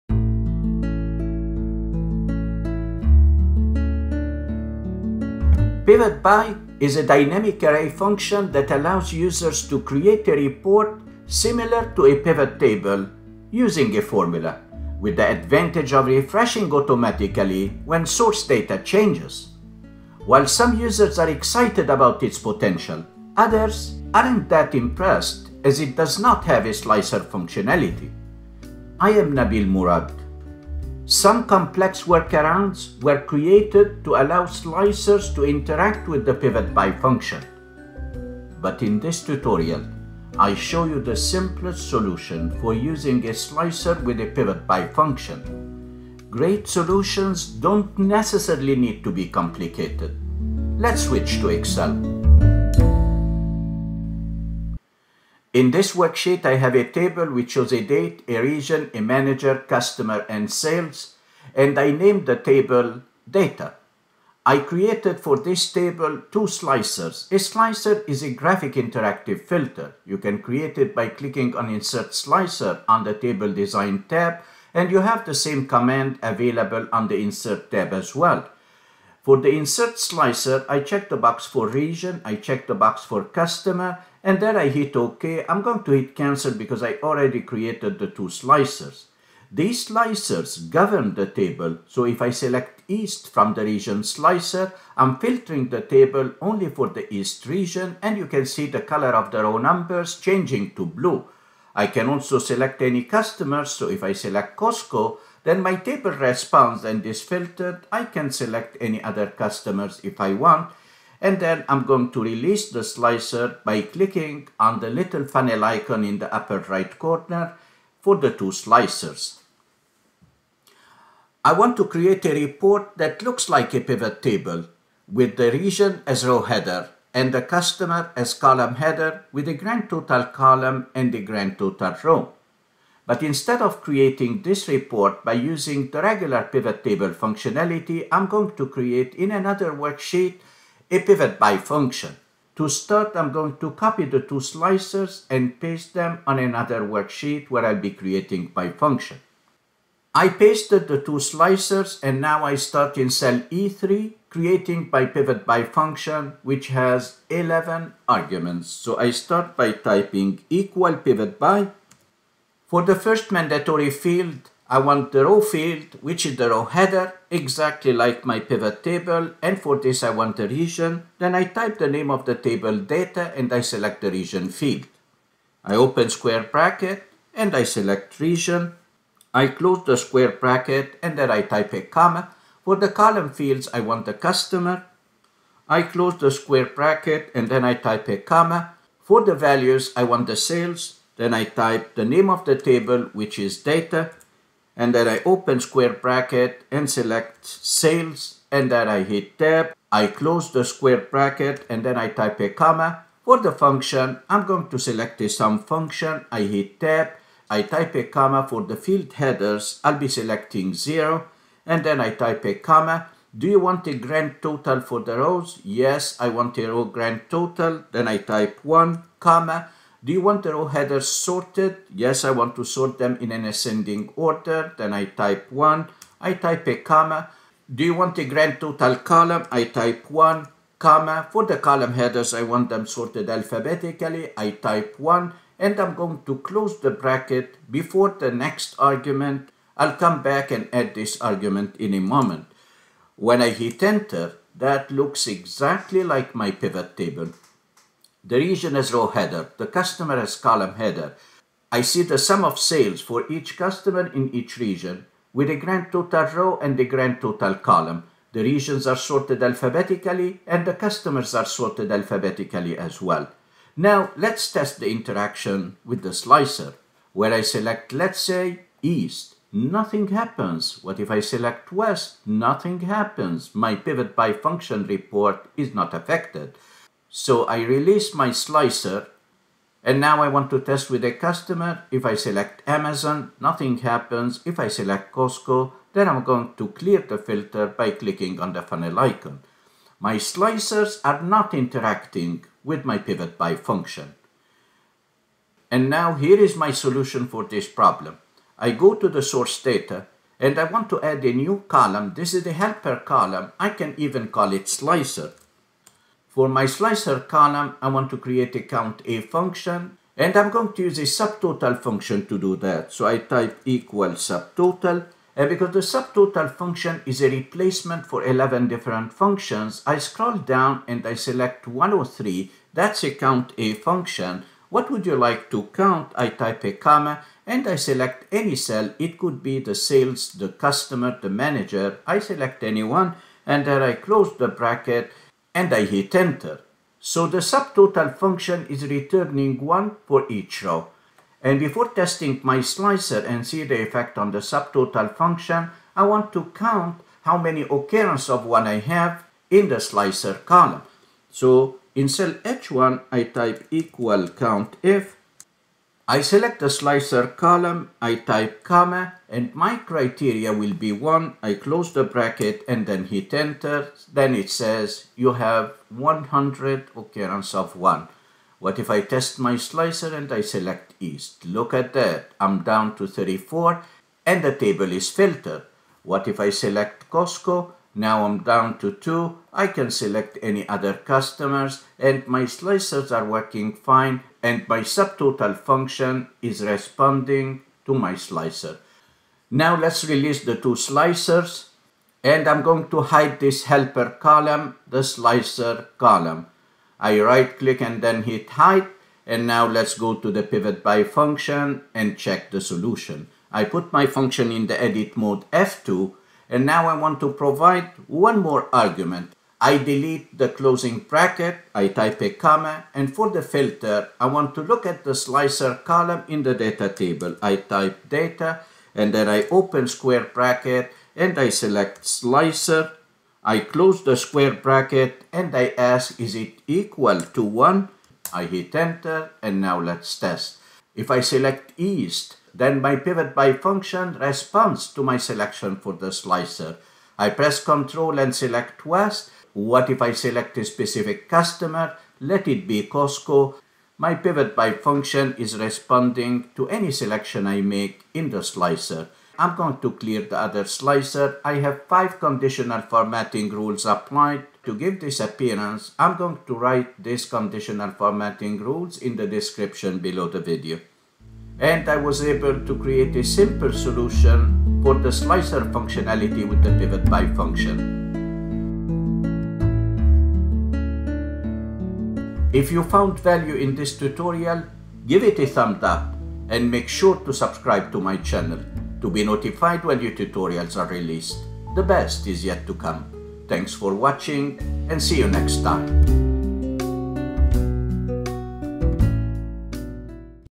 Simplest Solution For A Slicer With PIVOTBY Function
In November 2023, Microsoft introduced the PIVOTBY function via the Microsoft Insiders Beta channel. This dynamic array function enables users to create report-like summaries similar to pivot tables, with the benefit of automatically refreshing when the source data changes.
While some users are excited about its potential, others feel it falls short—particularly due to the lack of built-in Slicer functionality. Though workarounds exist to integrate Slicers with PIVOTBY, they tend to be too complex for the average user.
In this tutorial, I’ll show you the Simplest way to use a Slicer with the PIVOTBY function, bridging the gap between PIVOTBY and traditional Pivot Tables. Great solutions don’t have to be complicated
So let’s dive into Excel and get started!
Here are the Conditional Formatting rules I created to simulate the appearance of a Pivot Table:
5 Conditional Formatting Rules